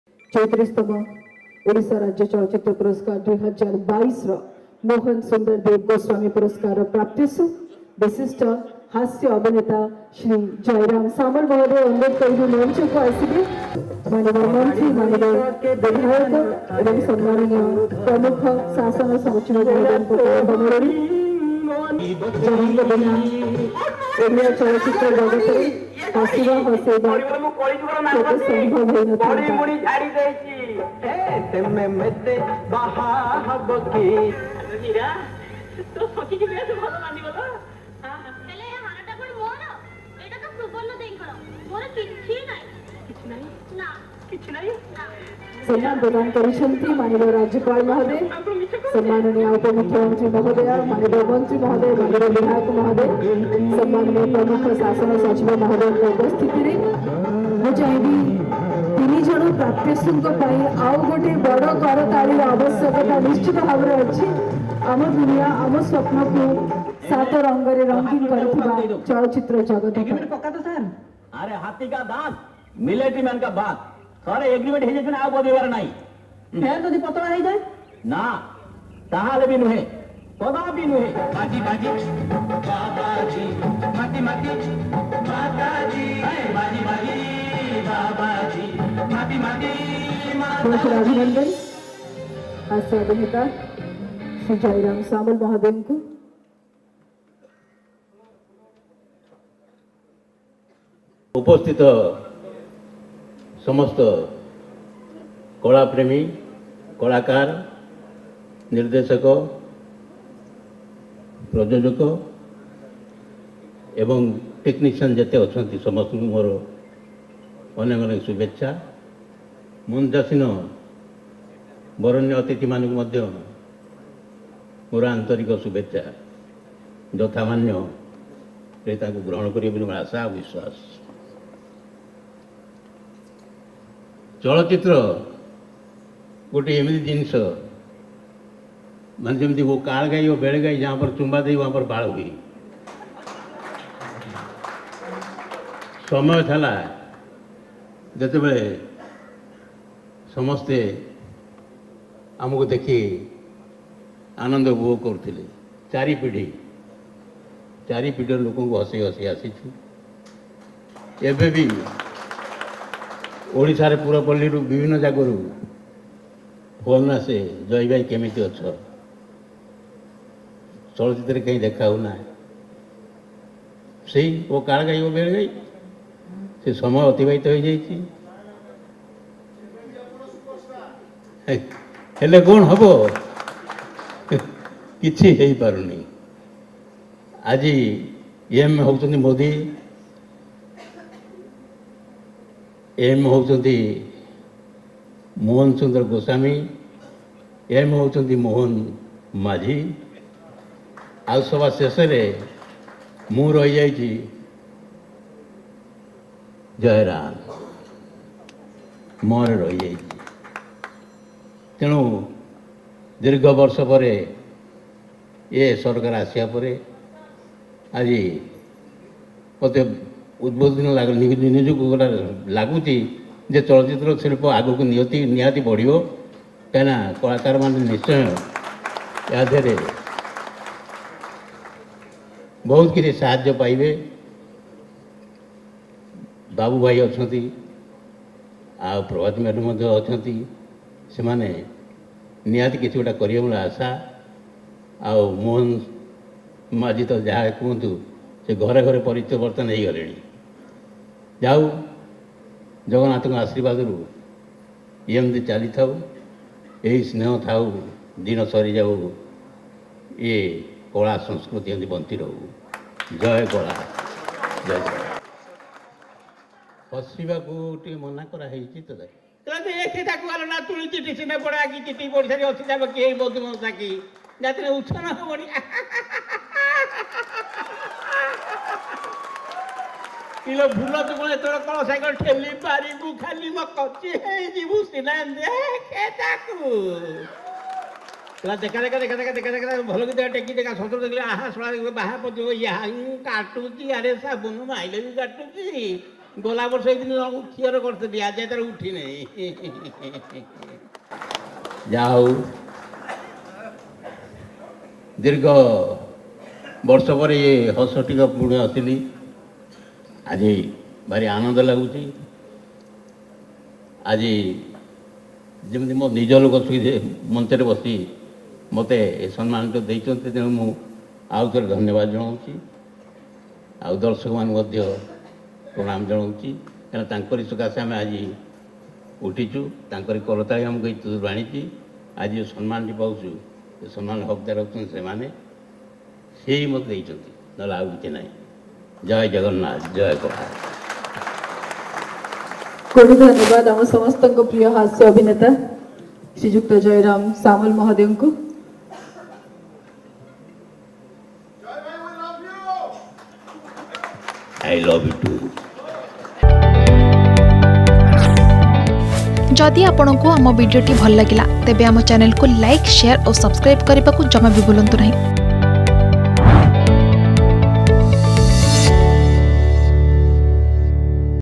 ମୋହନ ସୁନ୍ଦର ଦେବଗୋସ୍ବାମୀ ସମ୍ମାନରେ ସମ୍ମାନିତ ହେଲେ ହାସ୍ୟ ଅଭିନେତା ଜୟୀରାମ ସାମଲ | State Film Award
'ମୋହନ ସୁନ୍ଦର ଦେବଗୋସ୍ବାମୀ' ସମ୍ମାନରେ ସମ୍ମାନିତ ହେଲେ ବରିଷ୍ଠ ହାସ୍ୟ ଅଭିନେତା ଜୟୀରାମ ସାମଲ